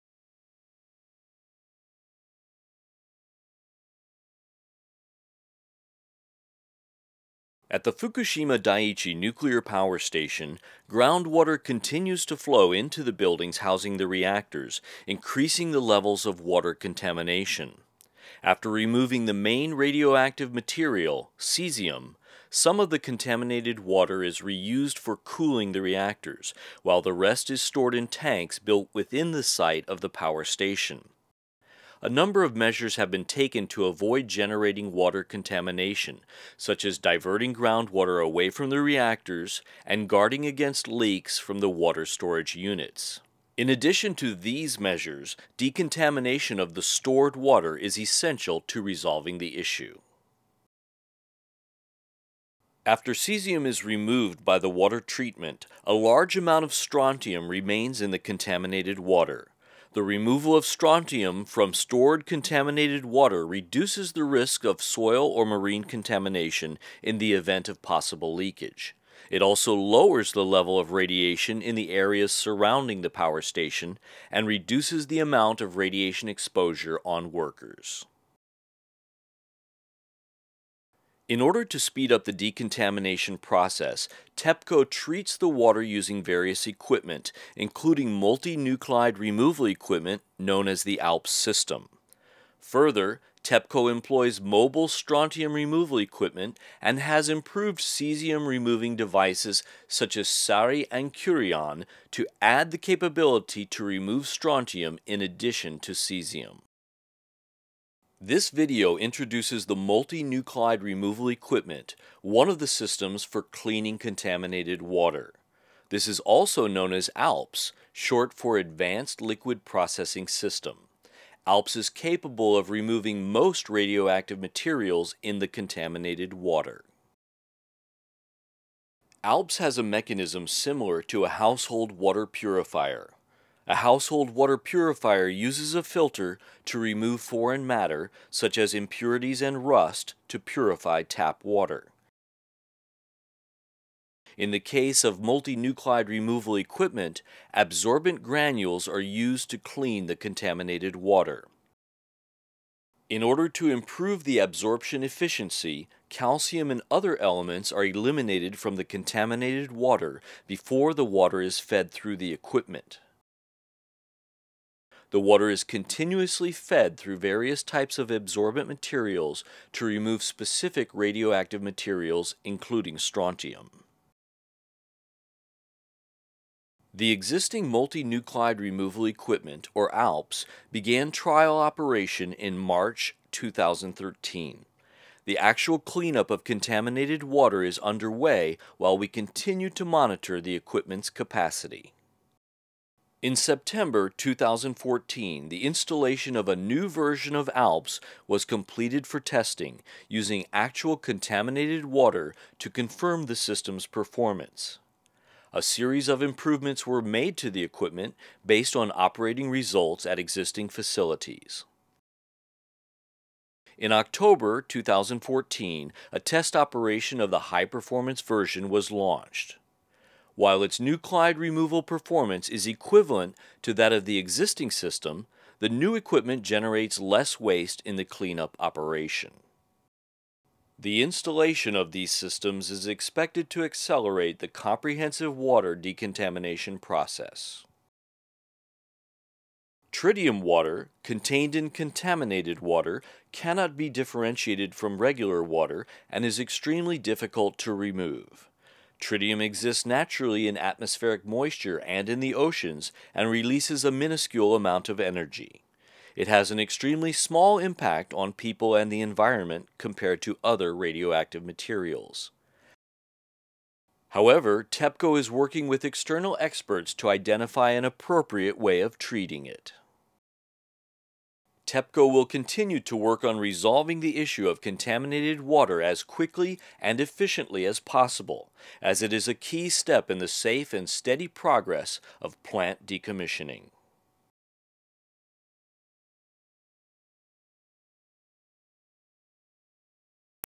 Contaminated Water Processing ～Multi-Nuclide Removal Equipment (ALPS)～
(04:50) In order to speed up the decontamination process at Fukushima Daiichi Nuclear Power Station, TEPCO treats stored contaminated water using various equipments, including ALPS, to add the capability to remove strontium in addition to cesium.